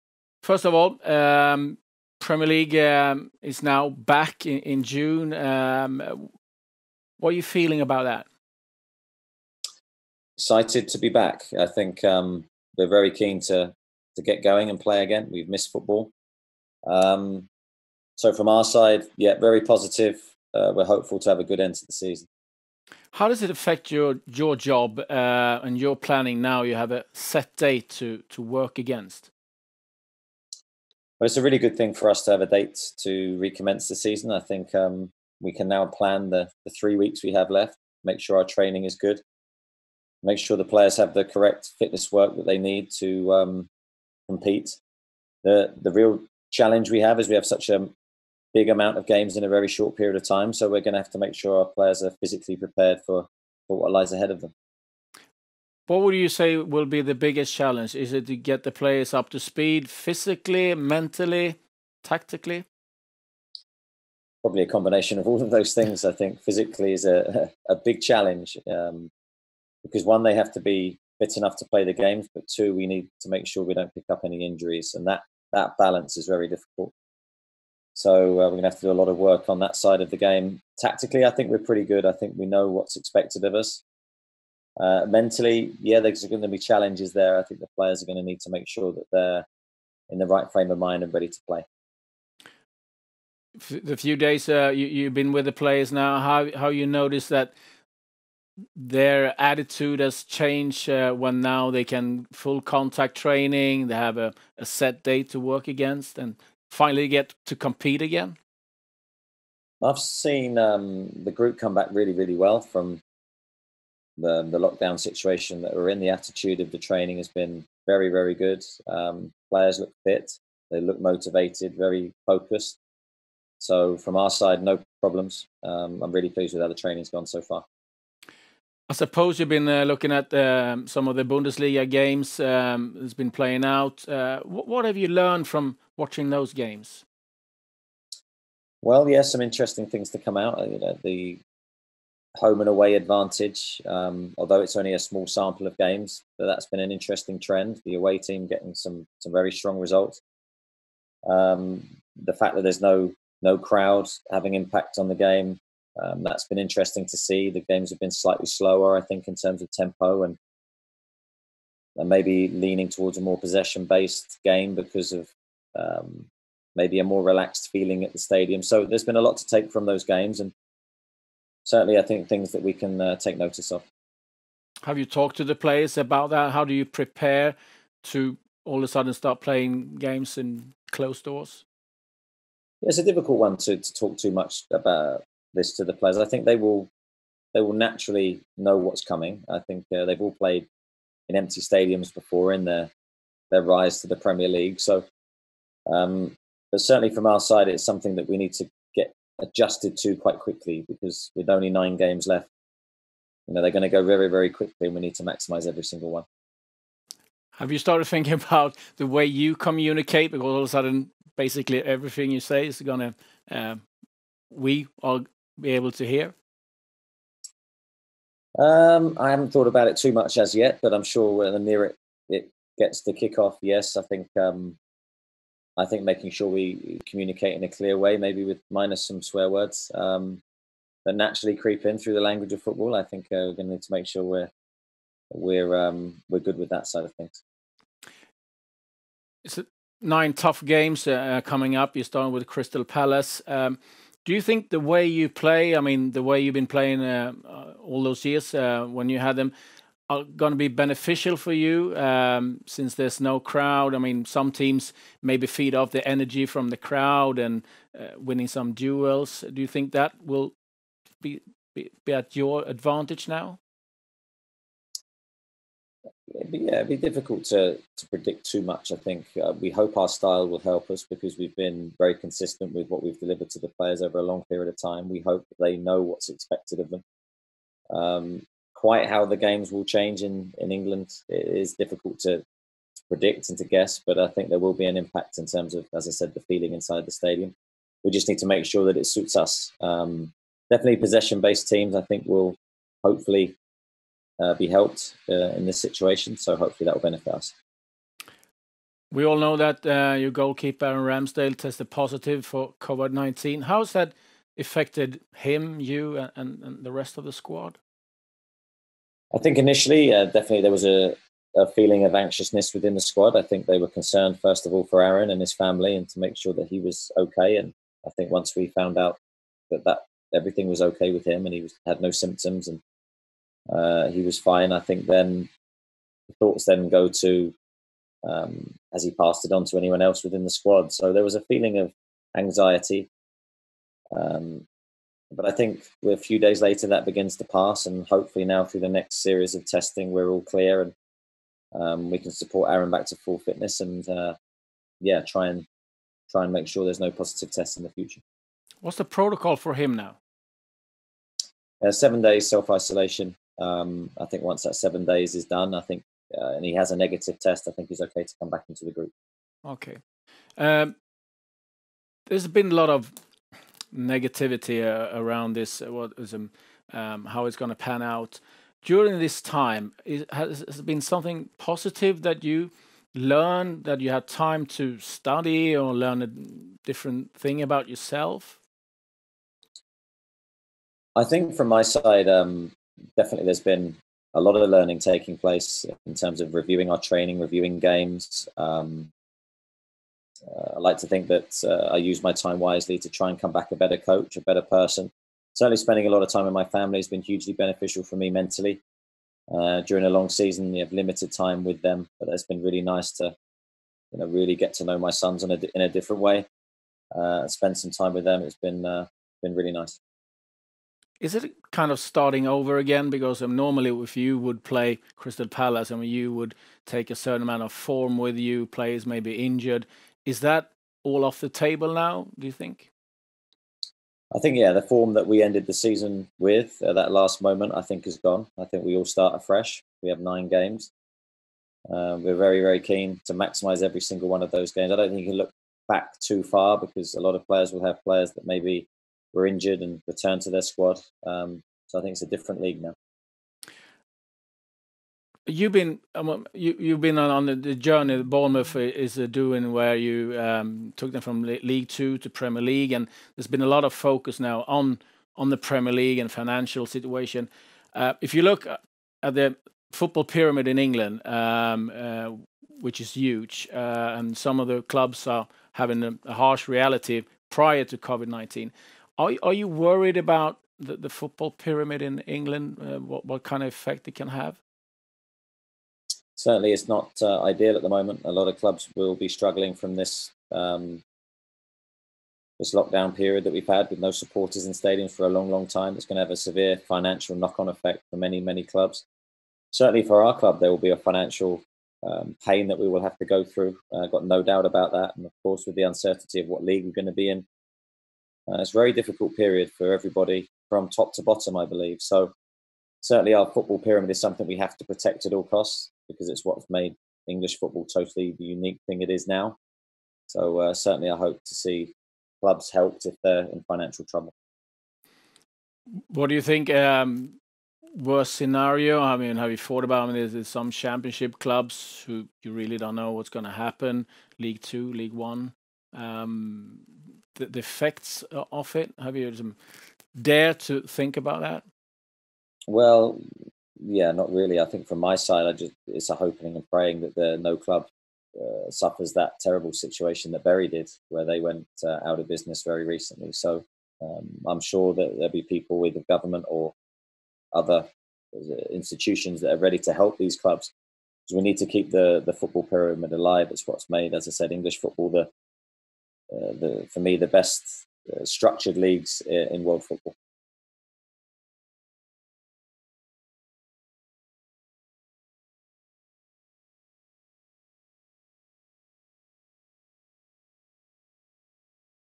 "We're all looking forward to getting back!" | Eddie Howe on Premier League return
Manager Eddie Howe discusses what the team have done differently and how the team are looking forward to getting back in this interview.